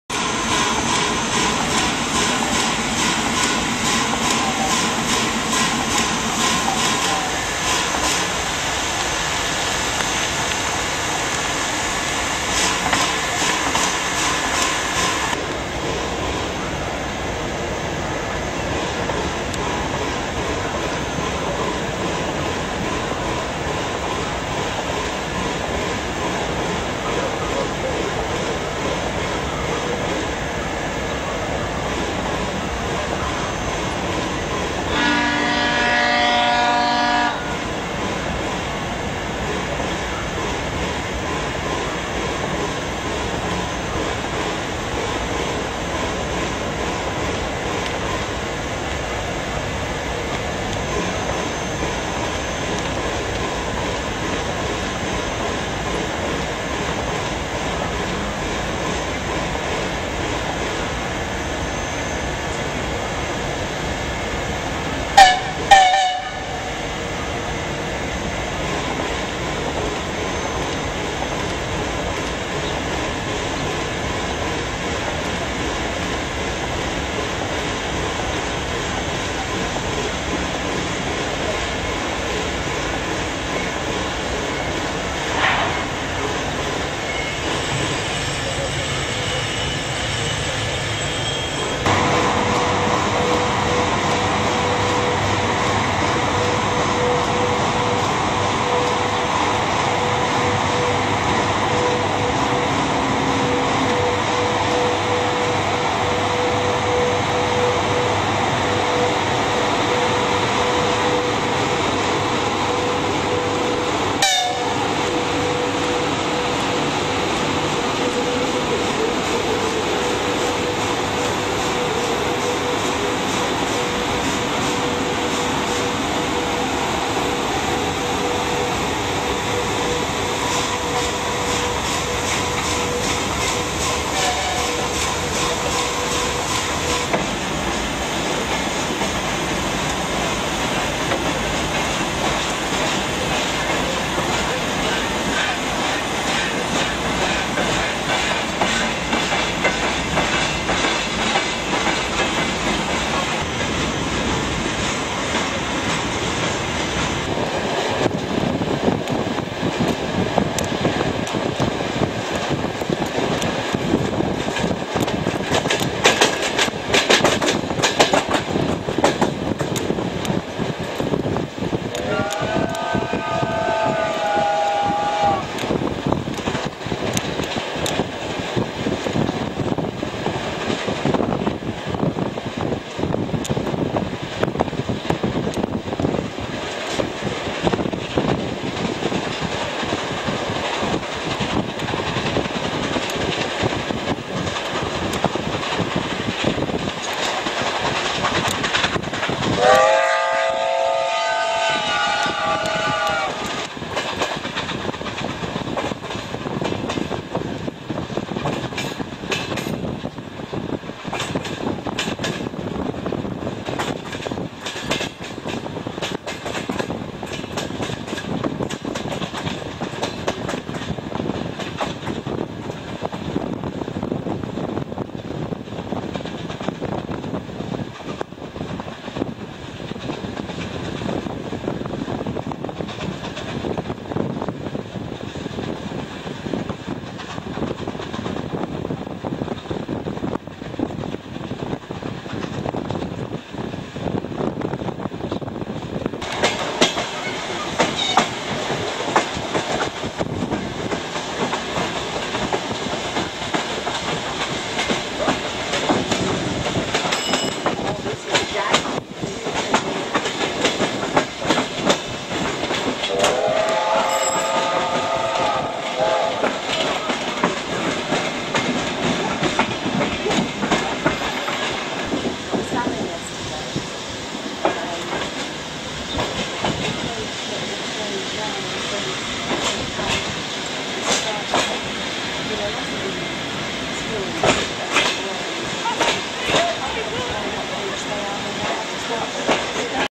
THE UKRAINE 2002 - Double headed steam in action
I was so busy filming at the station, I was just about left behind!